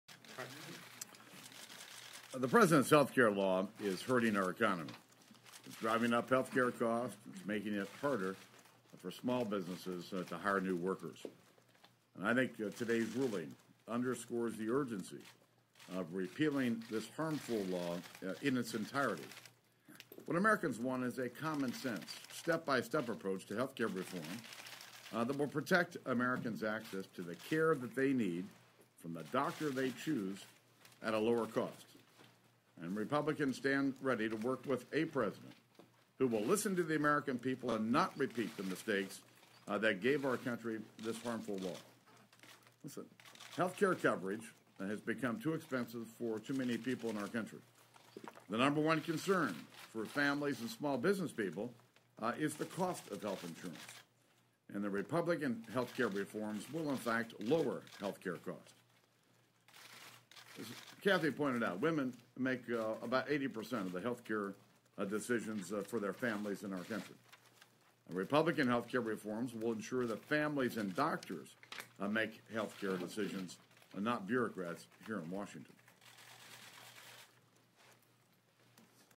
Speaker John Boehner Reacts to Supreme Court Decision Upholding Affordable Care Act
Follow NewsHour coverage of the Supreme Court decision

House Speaker John Boehner (R) reacts to the Supreme Court's early Thursday ruling upholding President Obama's Affordable Care Act.